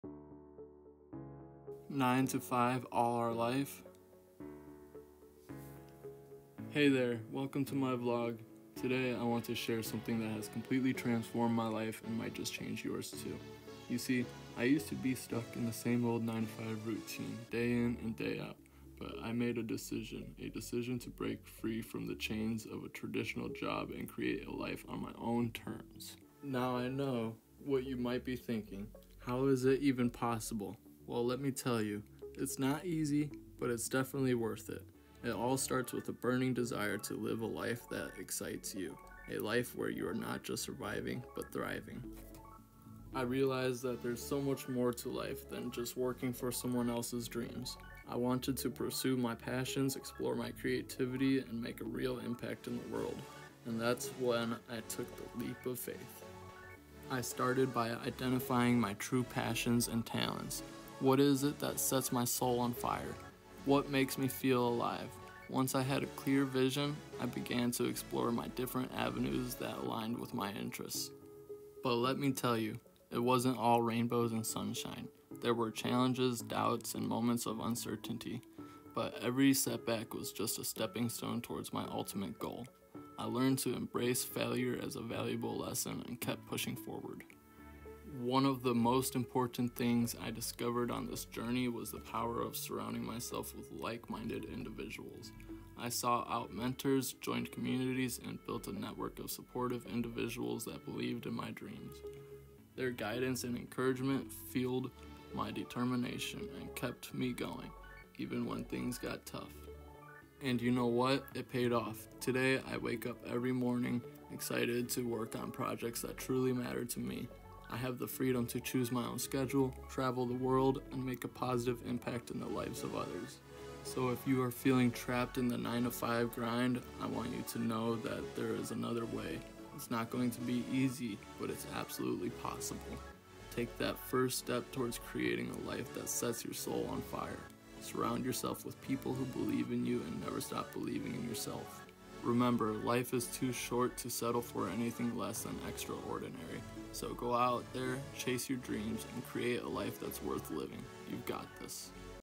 9-5 Job All Our Life?
In This Video I talk about how a 9-5 is Mediocrity & it builds other’s dreams, not ours ￼. In this video you might get a sense of direction which way you want to go in life￼. The easy road leads to the hard road but the easy road leads to the hard road, the choice is ours.